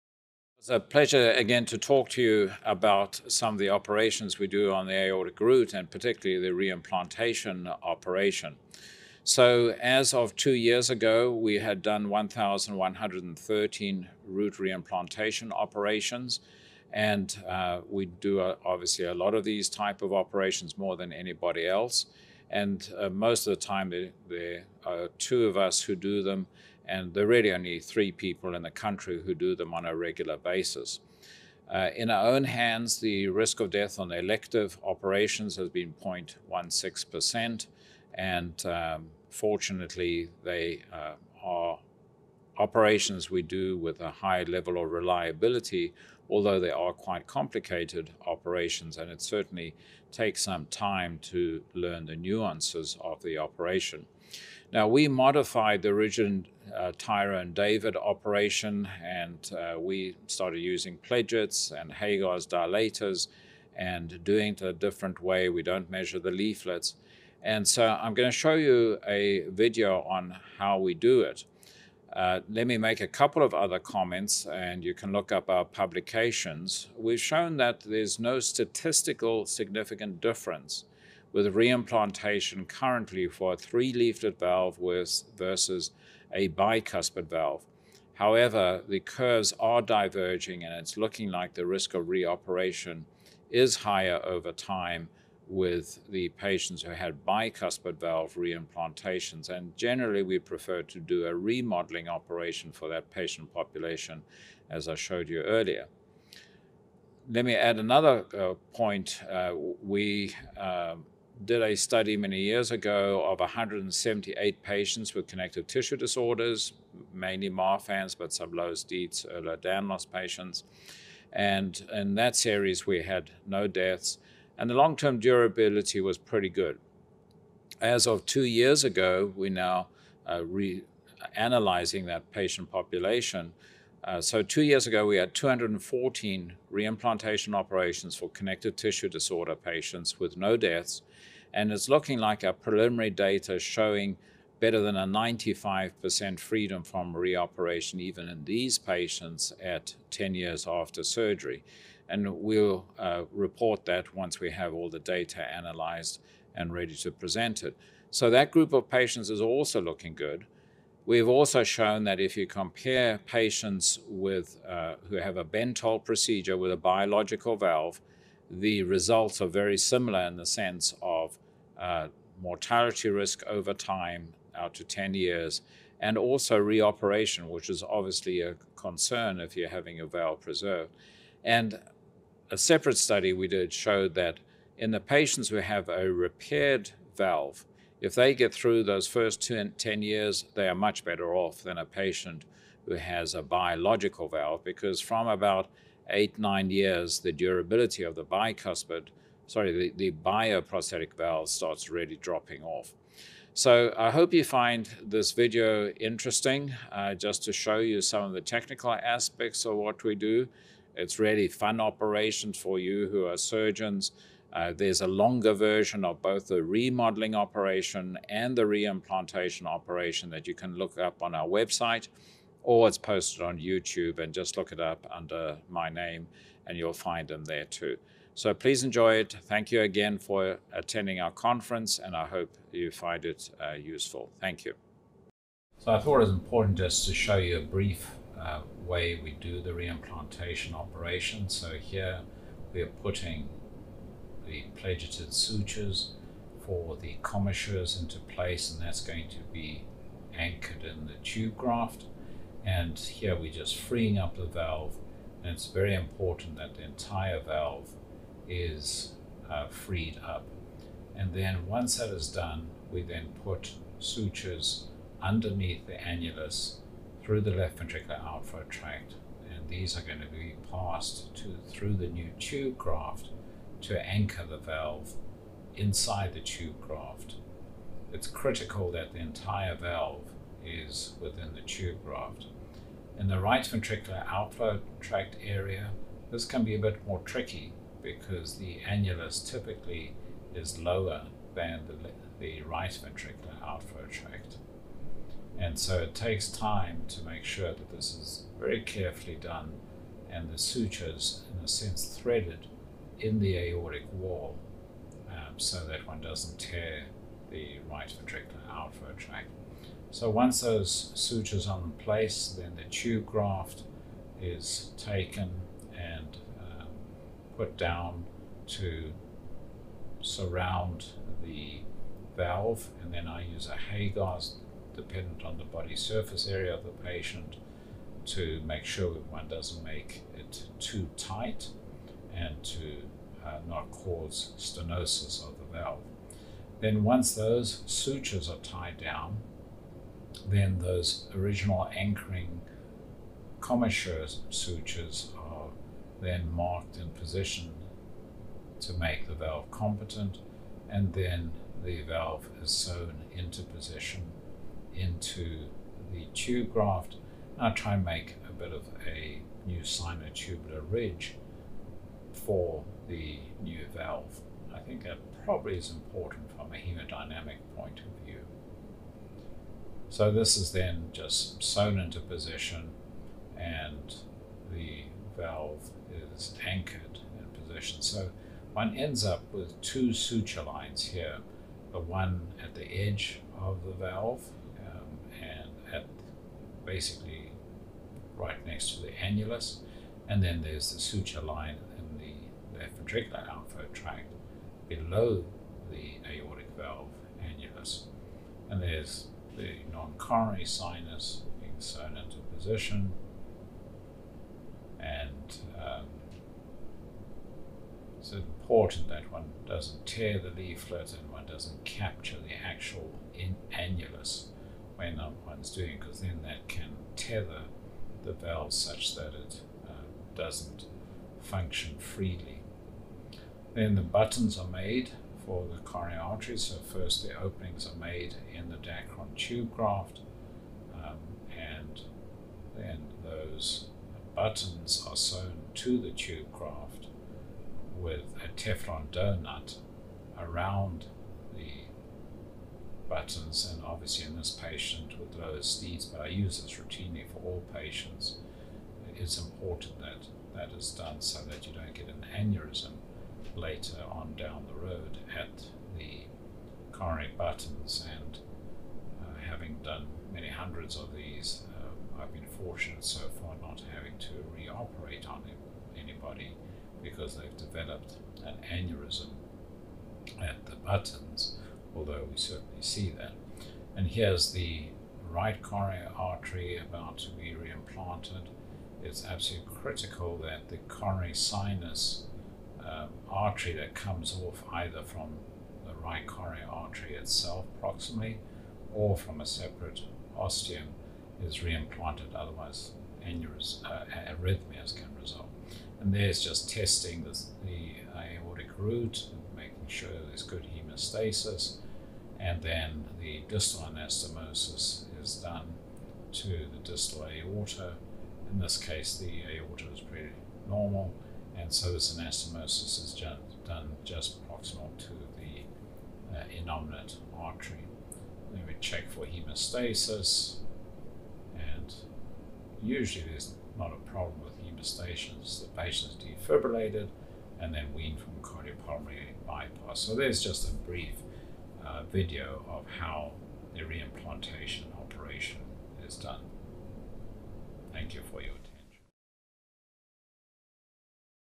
Valve Reimplantation (Graphic)
Warning: This video contains actual surgical footage which may not be suitable for all viewers.

Valve reimplantation is a surgical procedure performed on the aortic root.  Lars Svensson, MD, PhD and Delos M. Cosgrove M.D. describes the procedure performed at Cleveland Clinic and the outcomes.  He then demonstrates the aortic valve reimplantation surgical procedure.

Dr. Svensson is the Chairman of Cleveland Clinic's Heart, Vascular & Thoracic Institute.
Dr. Cosgrove is the Chair for Heart Disease Research.